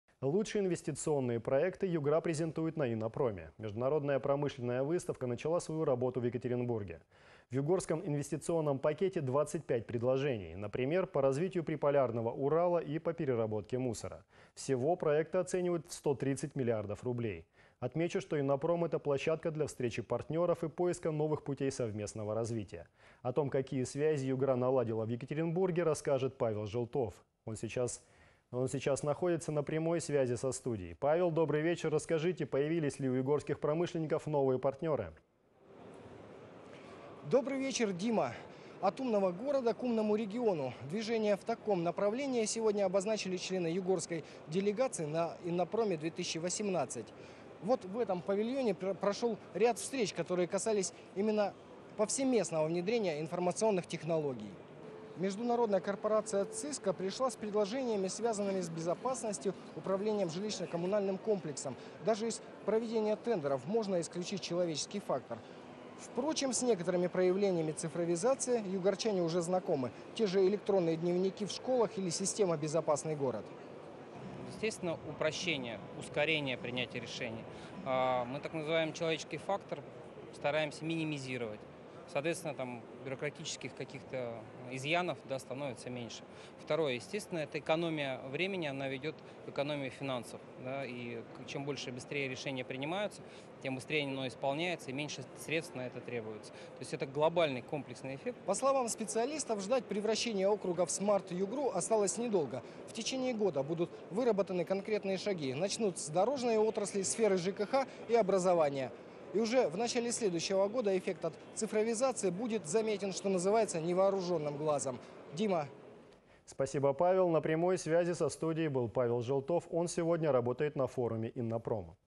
Международная корпорация сделает жизнь югорчан безопаснее
Лучшие инвестиционные проекты Югра презентует на «Иннопроме». Международная промышленная выставка начала свою работу в Екатеринбурге. В югорском инвестиционном пакете 25 предложений, например, по развитию приполярного Урала и по переработке мусора. Все проекты оценивают в 130 миллиардов рублей.